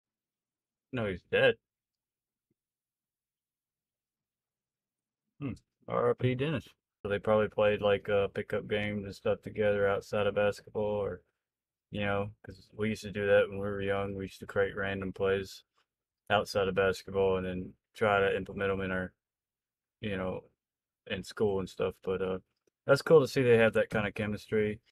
Have you ever created you're own play in basketball when you were little??
Dennis Johnson To Larry Bird Tribute--On Court Chemistry!

Yo Larry Bird and Dennis Johnson had the best chemistry i have ever seen!!